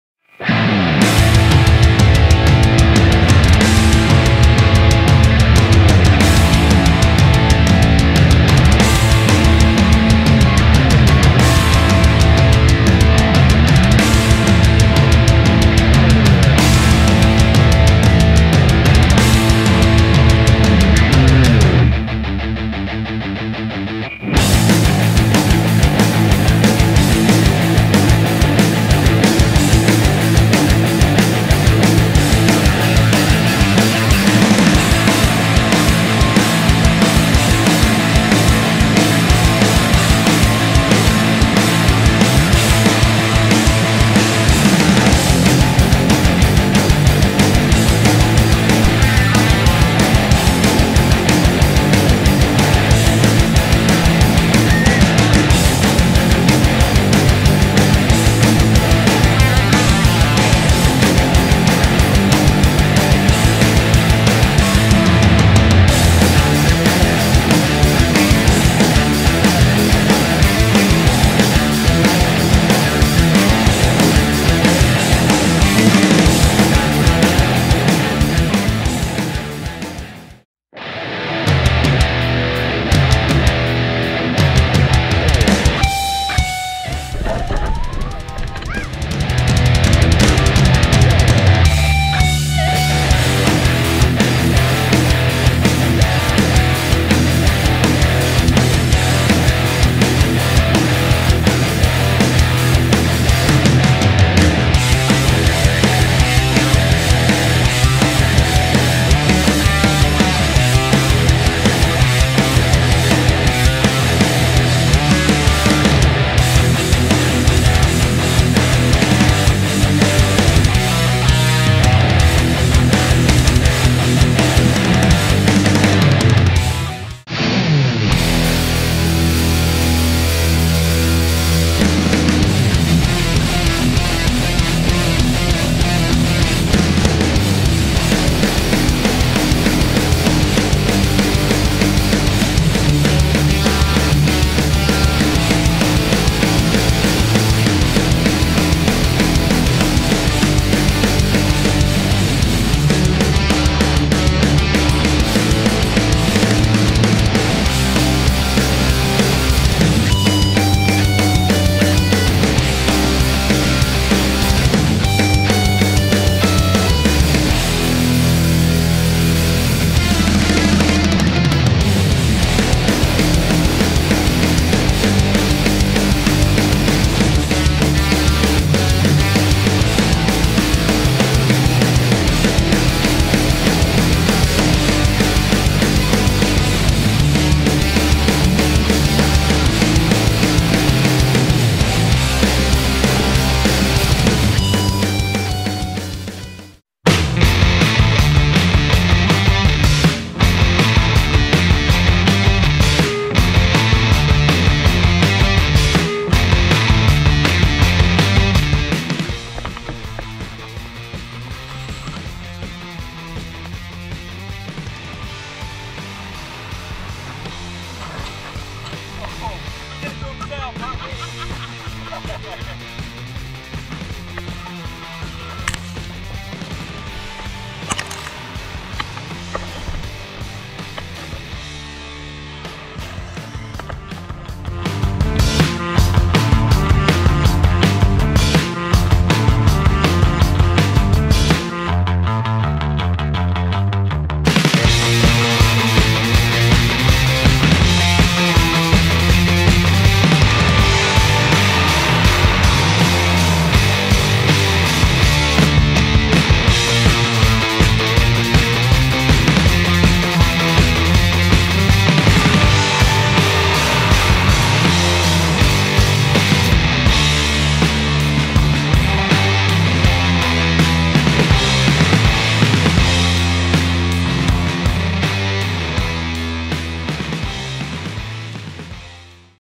Axial Yeti Trophy Truck & Traxxas Ford Fiesta ST Rally Action OMGRC
Axial Yeti Trophy Truck Driver: Bryan
Traxxas Ford Fiesta ST Rally  Driver: Joe
Traxxas is modified with a brushless hobbywing system on 3s